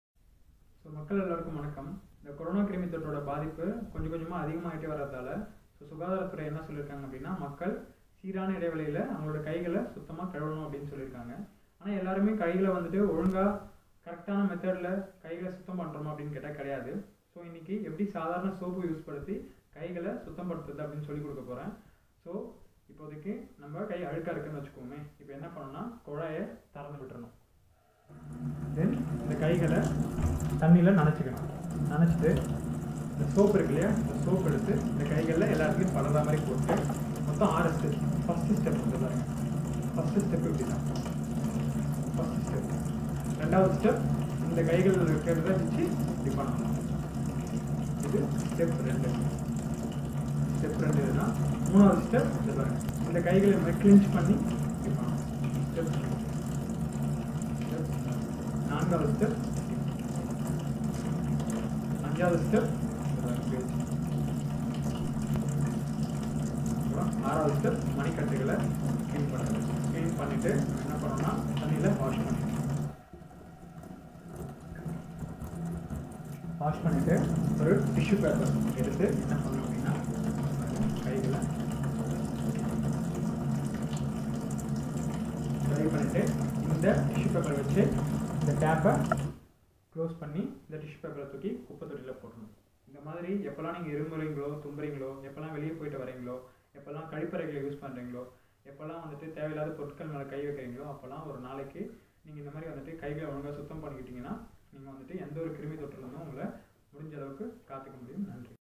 கொரோனா கிருமி நம்மை தாக்காமல் இருக்க ஒழுங்காக கை கழுவுவது எப்படி ?? 1 நிமிட கை கழுவும் முறை !!! 🙌
கொரோனா கிருமி நம்மை தாக்காமல் இருக்க ஒழுங்காக கை கழுவுவது எப்படி ??

1 நிமிட கை கழுவும் முறை !!!